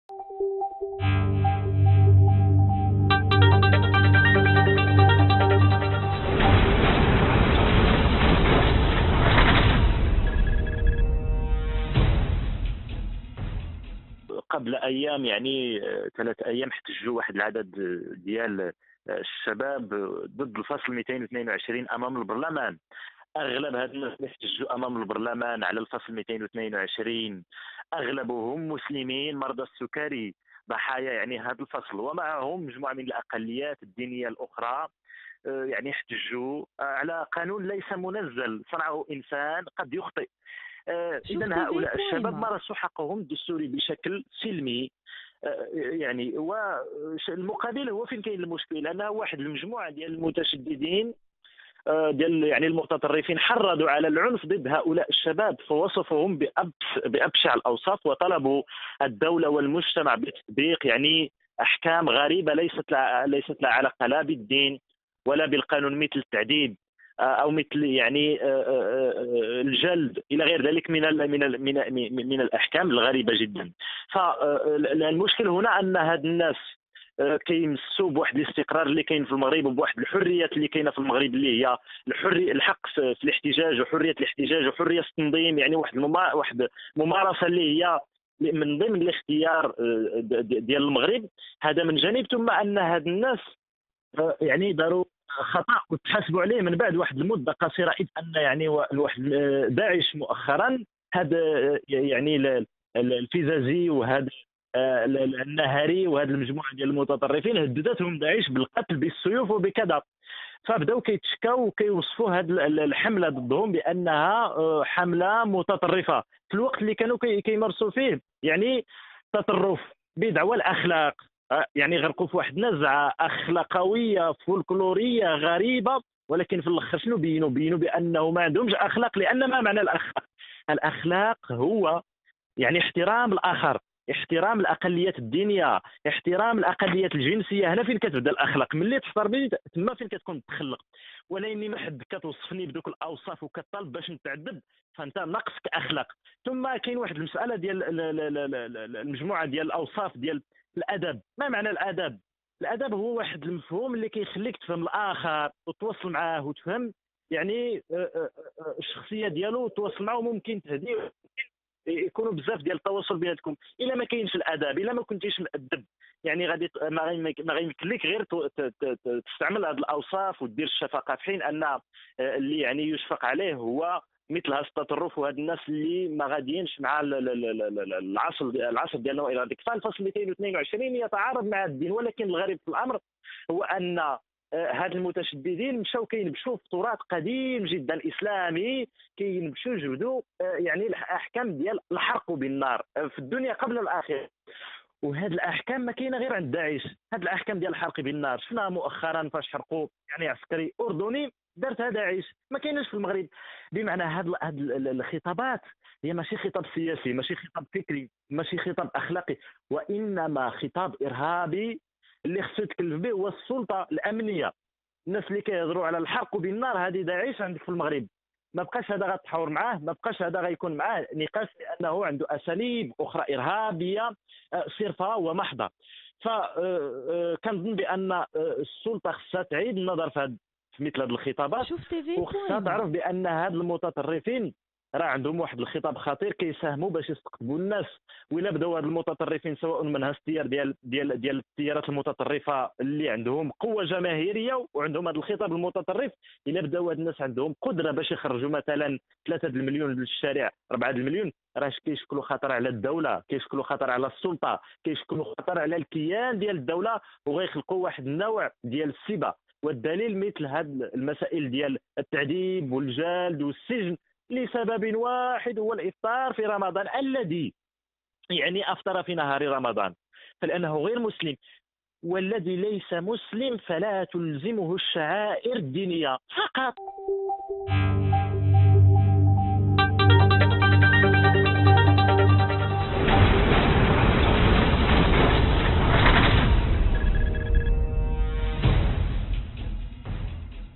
حركة تنوير ترد على تصريحات دعاة ''تعذيب وجلد وحرق'' مفطري رمضان | شوف تيفي
حركة تنوير ترد على تصريحات دعاة ''تعذيب وجلد وحرق'' مفطري رمضان | شوف تيفي 2016